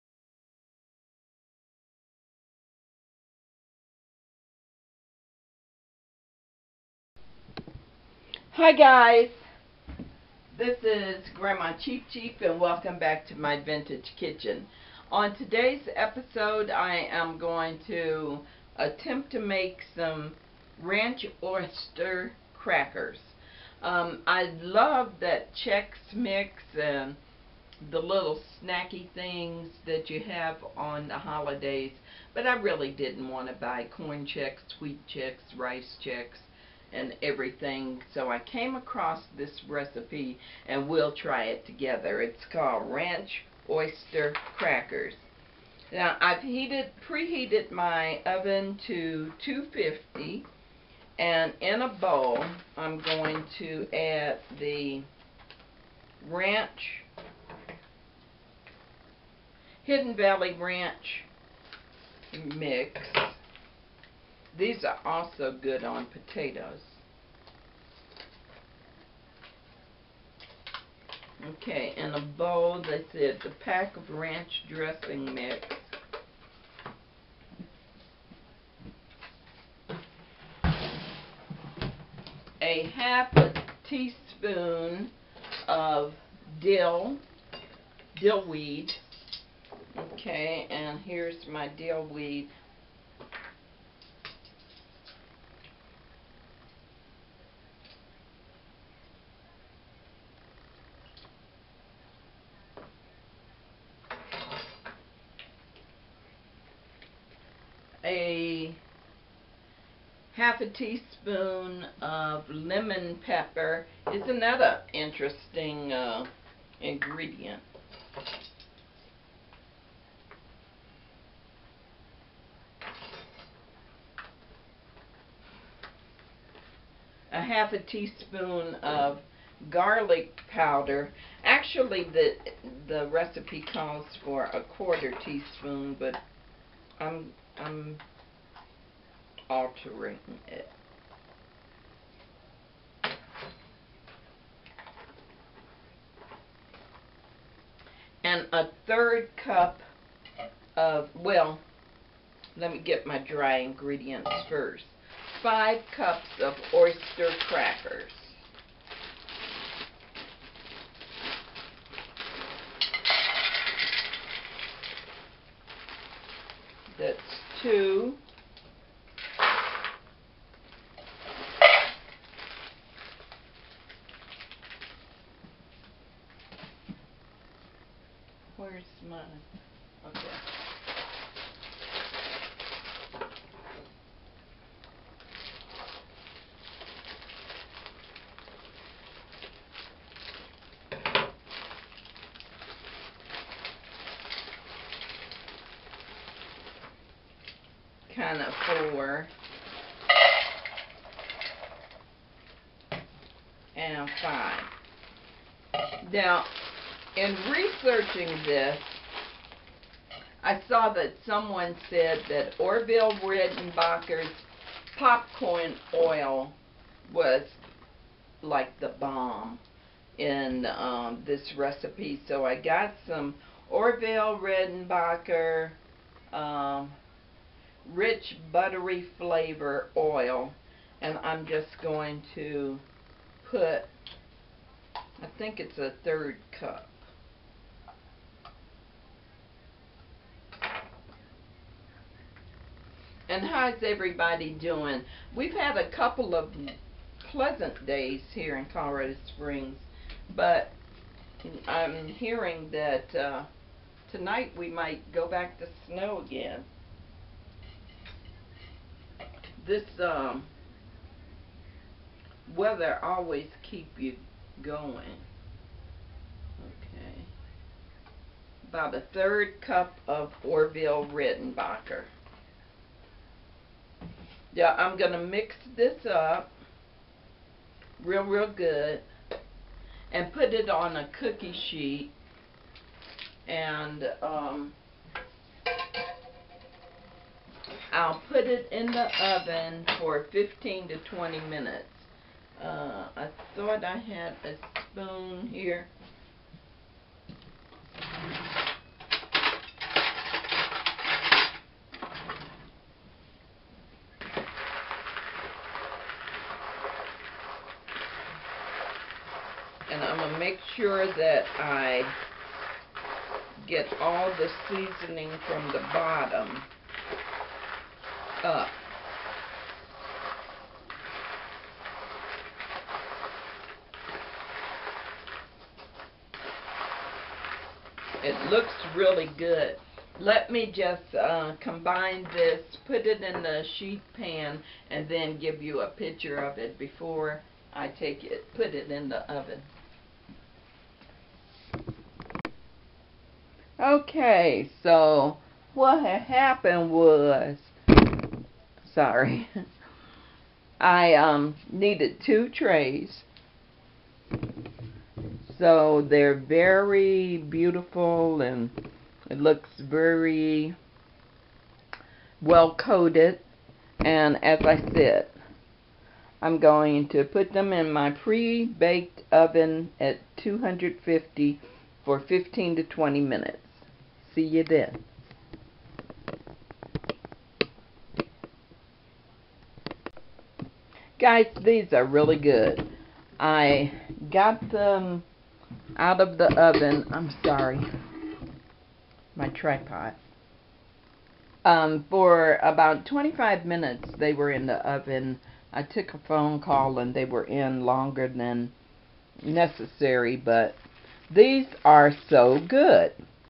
Ranch Oyster Crackers
1 pkg Hidden Valley Ranch style dressing mix
1/2 tsp dried dill weed
1/2 tsp lemon pepper
1/2 tsp garlic powder
5 cups Oyster Crackers
1/3 c Orville Redenbachers Butter Popcorn Oil

Preheat oven to 250 degrees.   In a large bowl, combine all your dry ingredients including crackers.  Pour oil in the bowl and stir making sure all crackers are covered in oil mixture.

Bake 25 minutes and allow to cool before serving.

DELICIOUS